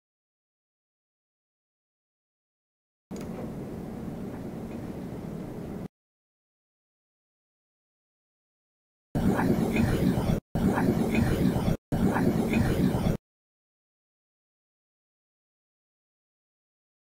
EVP caught while reviewing evidence
Is this saying "I don't care to" or "I will kill you"? What do you think?

I caught this while I was reviewing evidence from another case...